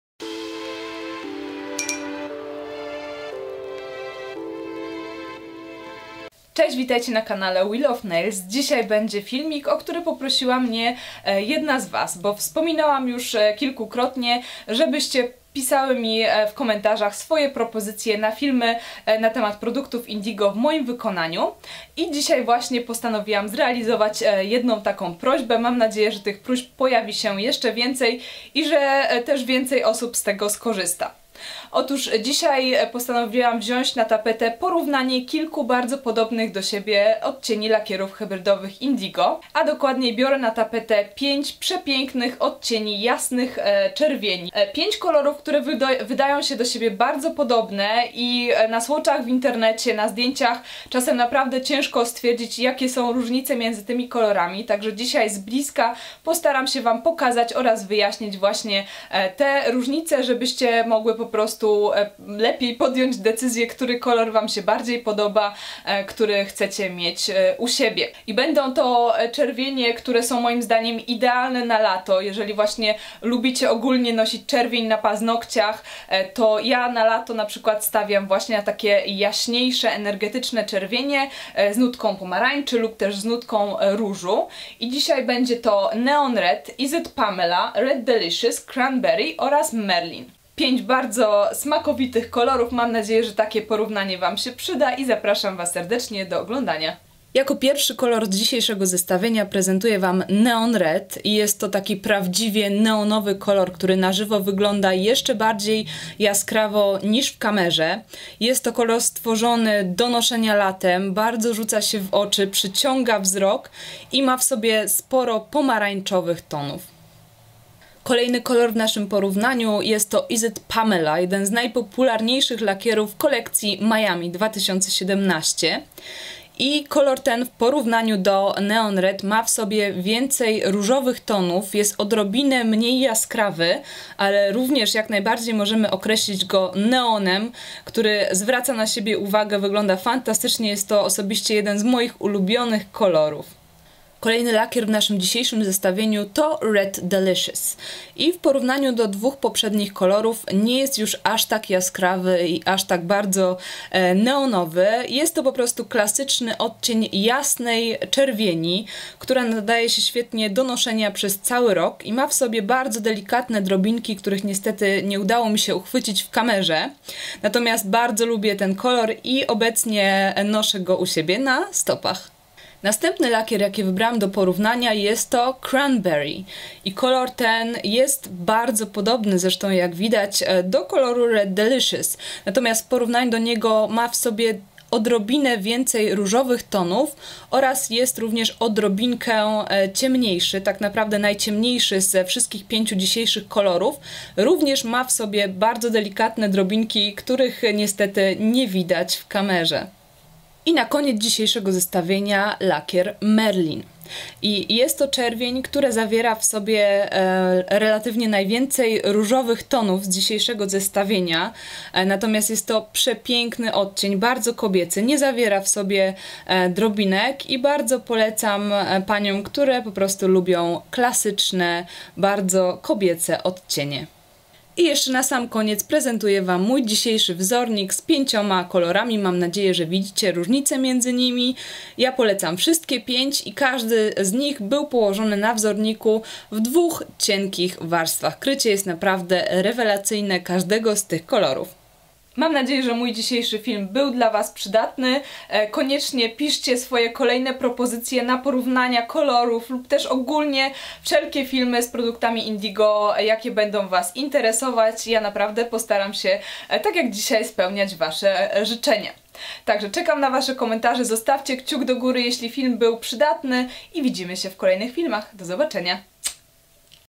PORÓWNANIE :: Jasne czerwienie Indigo :: Lakiery hybrydowe
Jasne czerwienie Indigo - którą z nich wybrać? Jakie są różnice?
 Zapraszam na porównanie kolorów! Dziś na tapetę biorę 5 zbliżonych do siebie odcieni lakierów hybrydowych Indigo. Będą tą kolory idealnie na letnią porę roku... i nie tylko :)

Nie chcesz przegapić nowości i trendów?

DZISIAJ MÓWIĘ O:

► Is it Pamela? Gel Polish